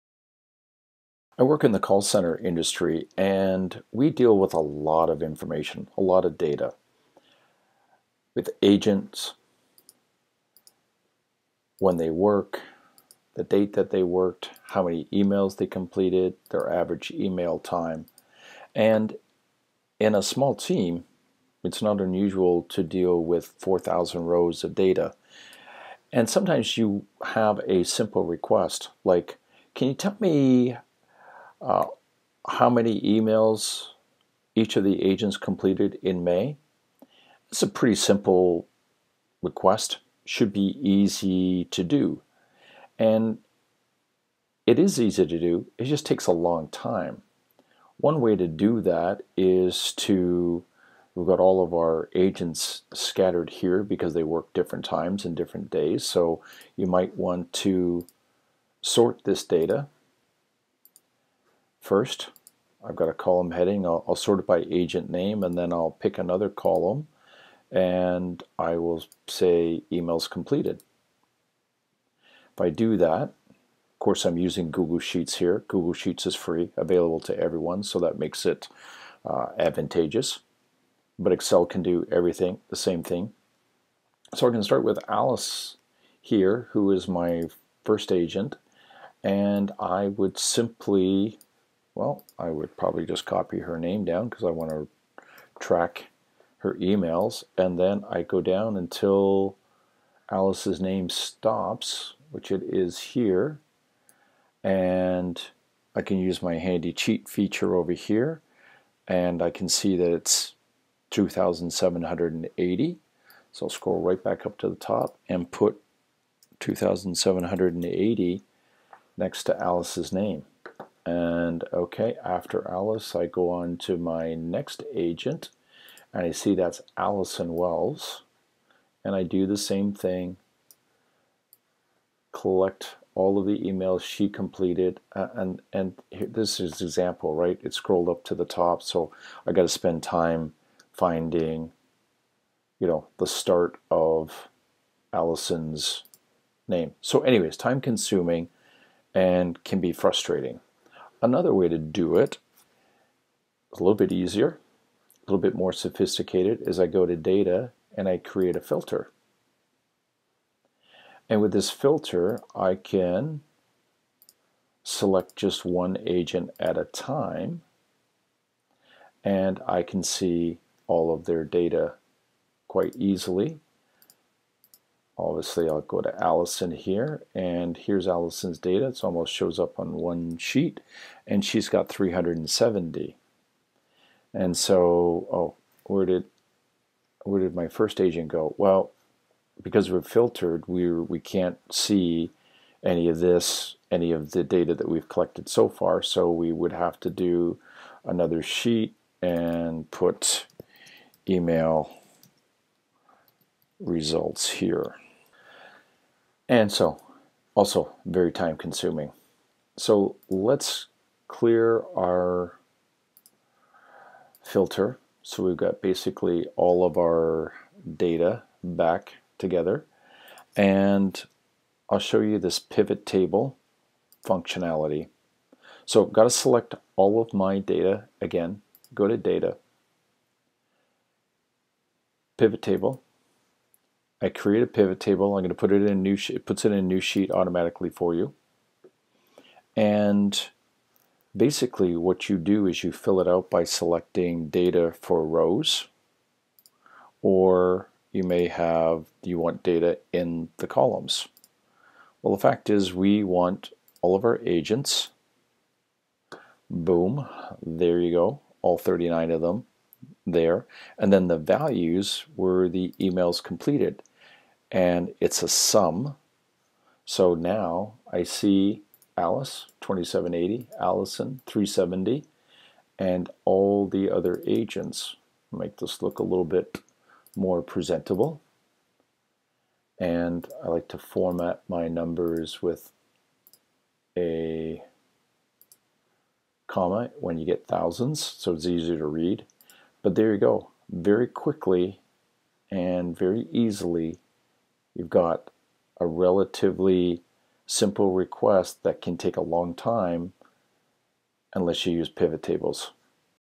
Save Hours of Spreadsheet Work with Pivot Tables
Say goodbye to Sorting and Filters and long hours trying to get the data you need from your spreadsheets.